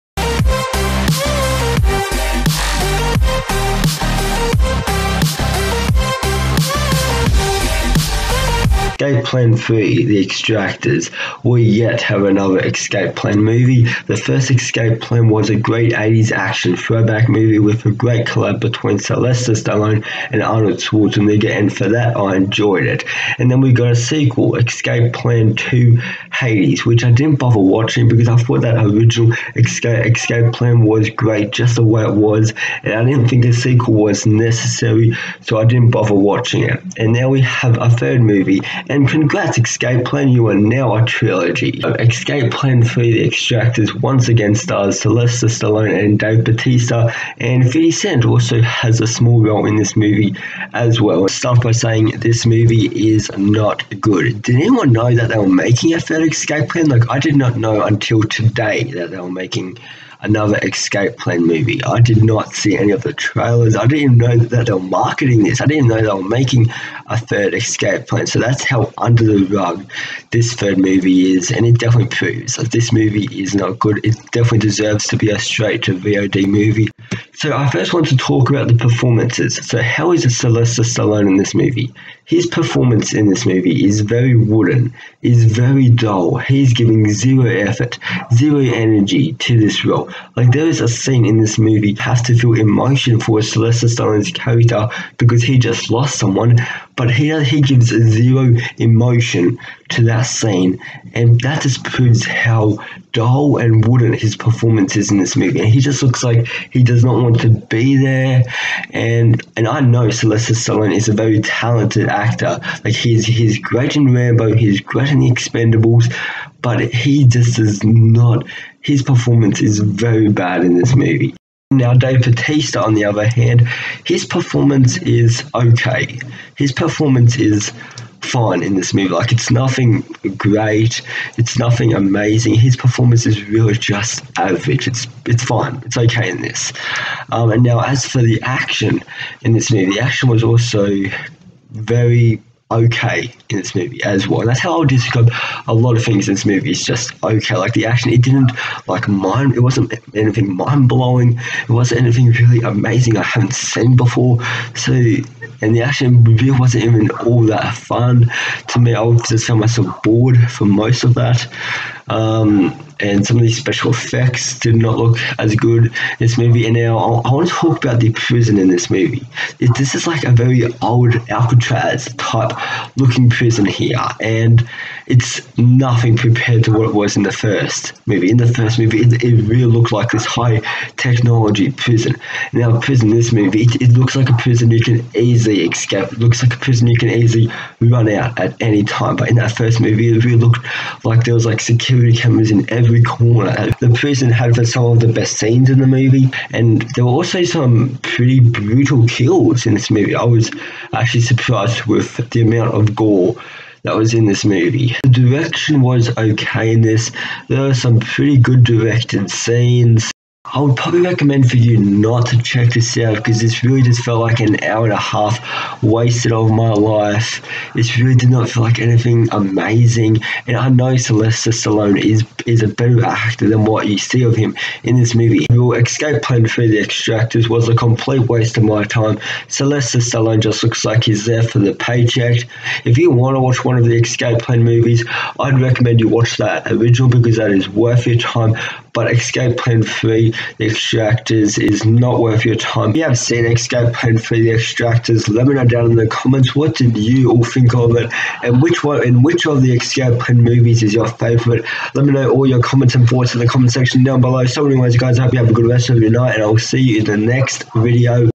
Escape Plan 3: The Extractors - Movie Review
Sylvester Stallone returns with Dave Bautista to kick some more ass in the 3rd installment in the  Escape Plan series. Escape Plan 3: The Extractors is Directed by John Herzfeld. Here are my thoughts on Escape Plan 3: The Extractors!